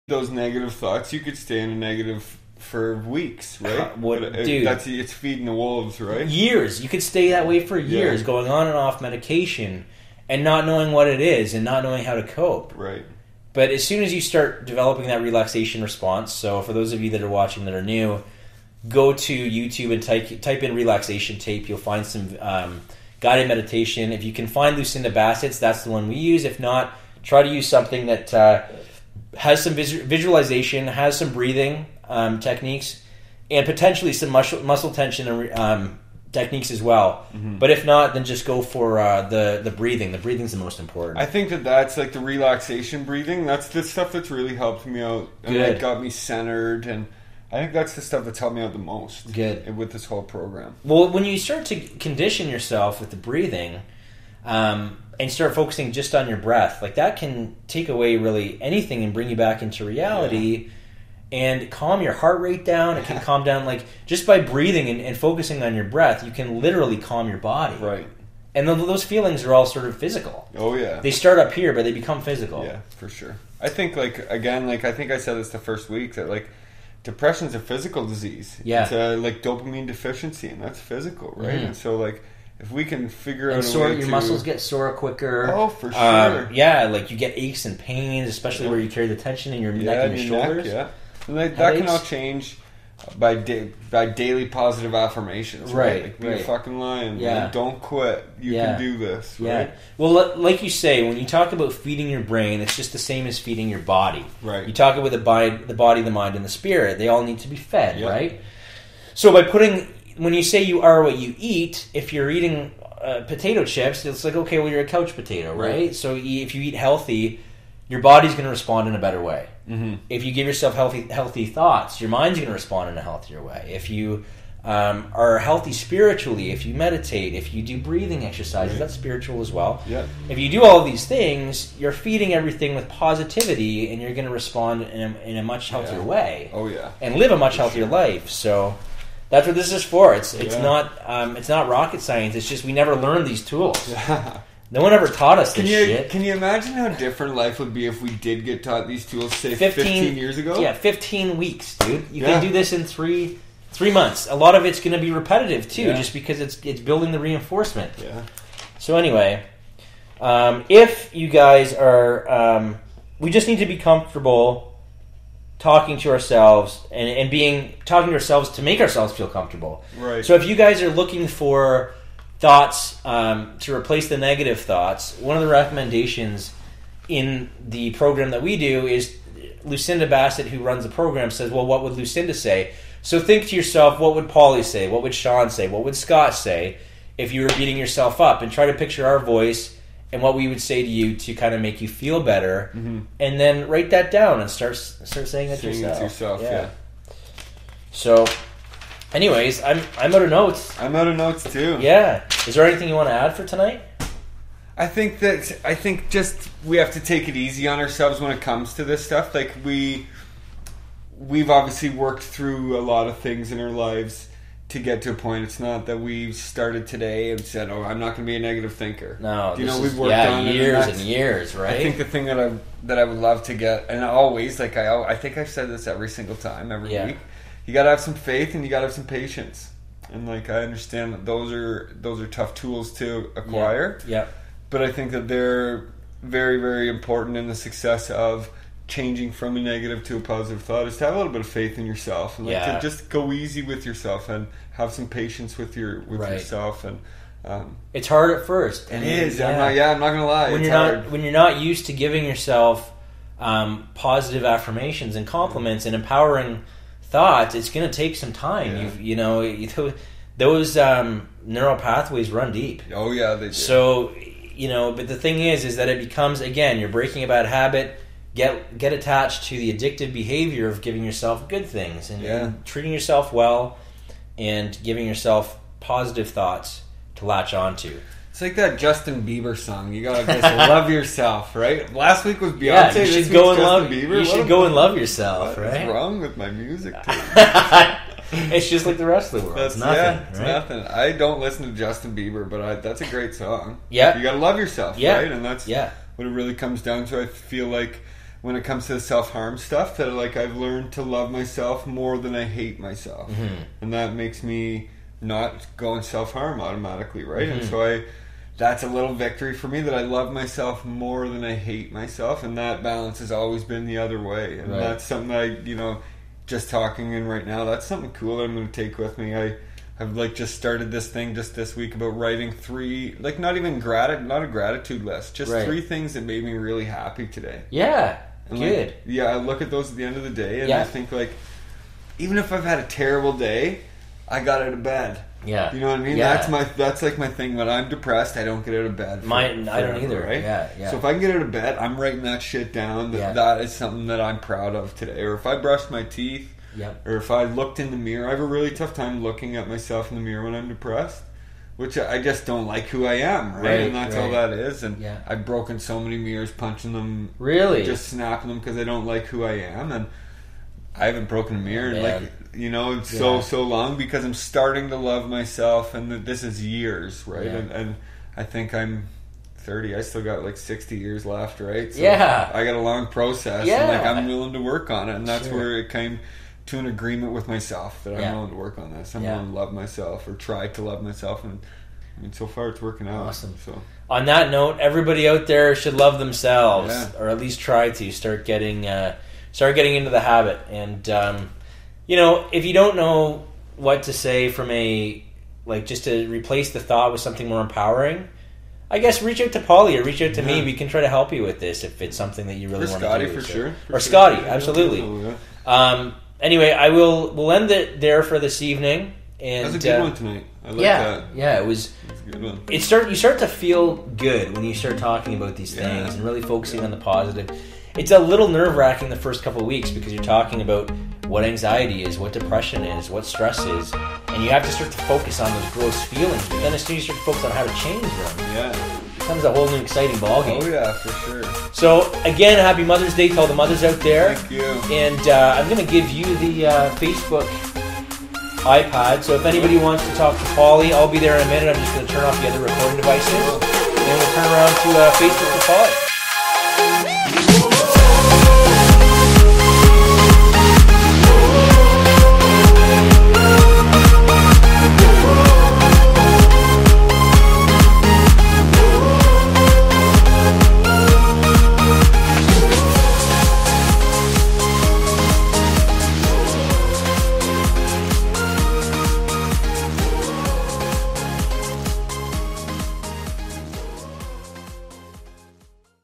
Sex & Suicide Podcast #28 - Soulfire Sunday Episode #4 Ft. Shawn Evans & Paulie O'Byrne (part 5 of 5
Welcome to episode 4 of the Sunday Mental Health, Cognitive Behavioral Therapy Program, where Paulie and Shawn talk about a very crucial component to recovering from anxiety, stress, and depression. Self-Talk: The Key To Healthy Self Esteem is the main topic covered in this session.